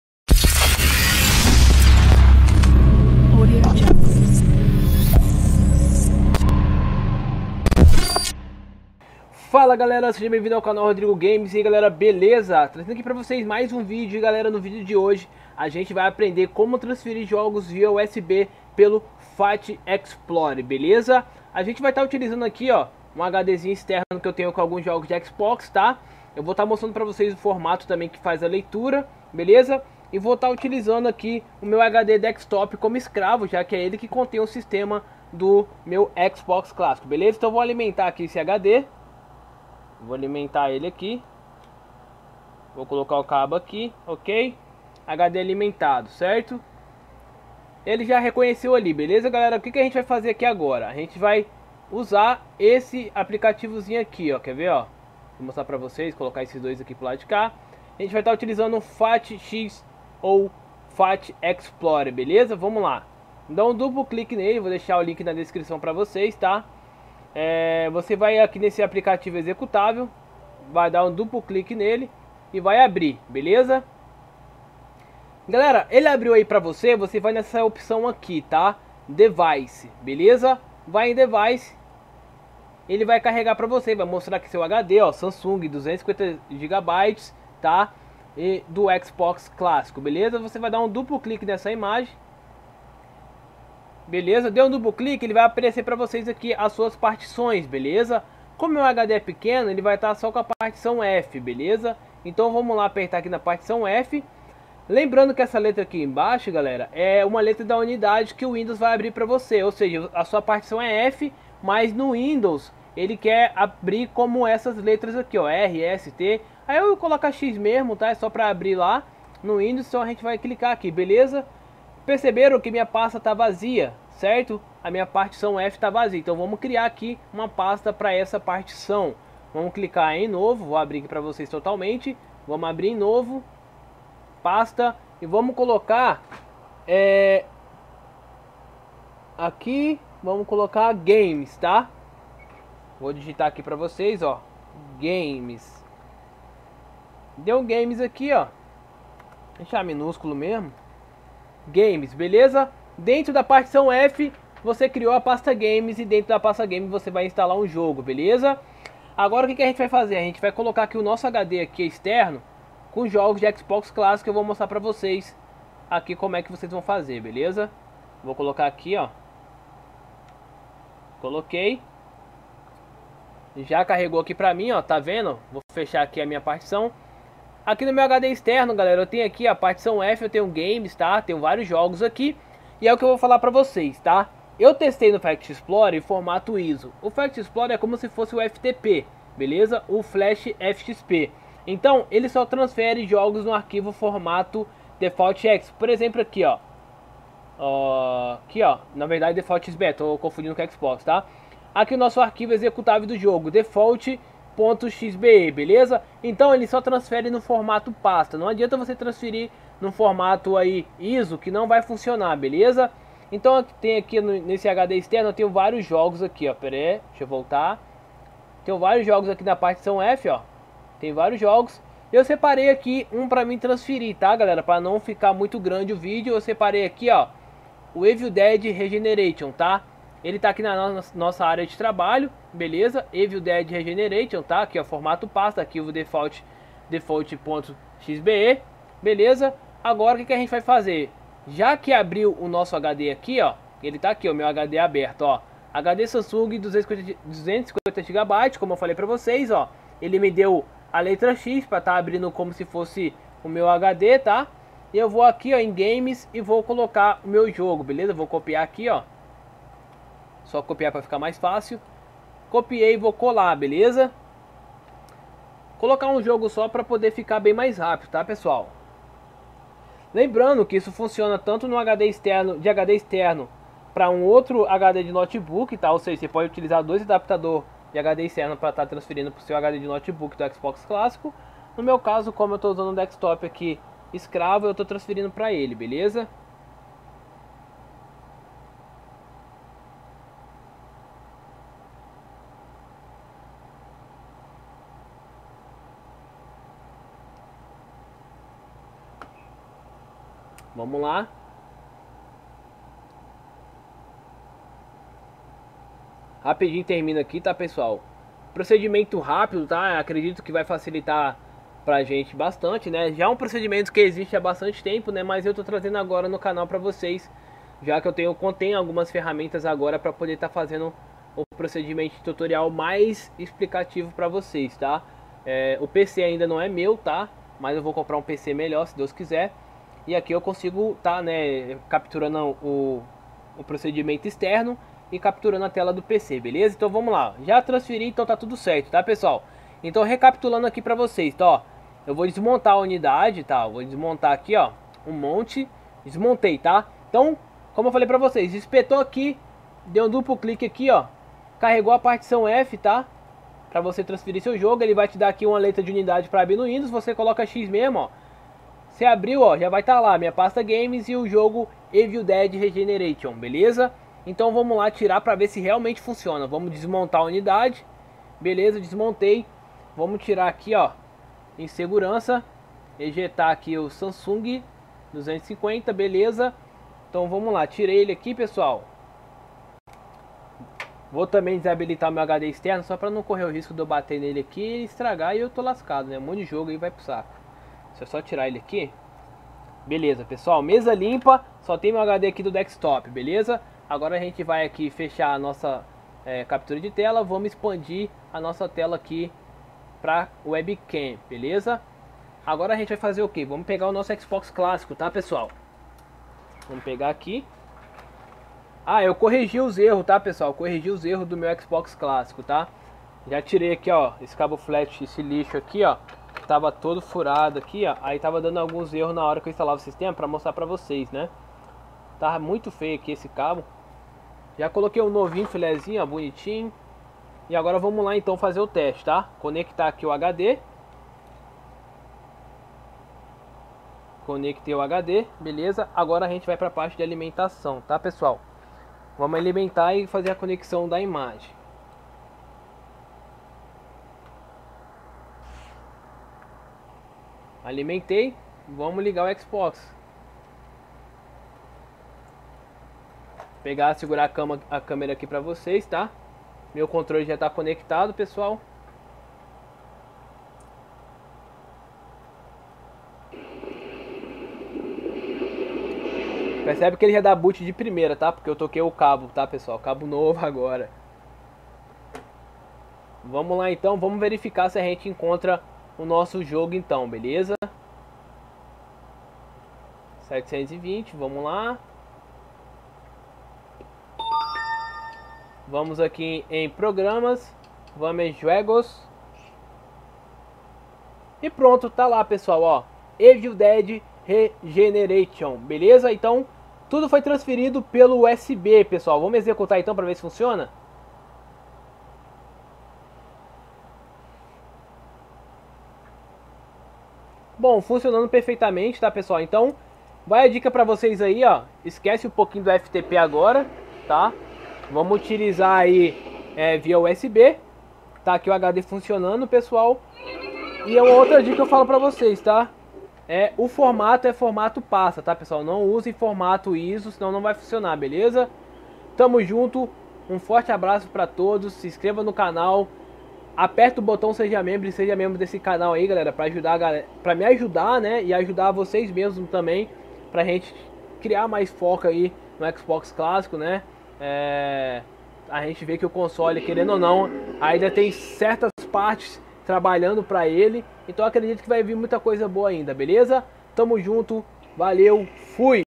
Transferindo jogos para o Xbox clássico via USB de forma Prática e rápida sem FTP/REDE!
Fala galera mais um vídeo para vocês, espero que gostem...
Agora de forma rápida e fácil conseguiremos transferir jogos para o hd do xbox clássico via USB!
✅Xbox clássico,
✅O melhor console,
✅ da 6° Geração!

▬▬▬▬▬▬▬🔻🔻 Links Importantes 🔻🔻▬▬▬▬▬▬▬▬▬▬▬▬

🔻🔻 Links Importantes 🔻🔻

✅Seja Gamer seja você!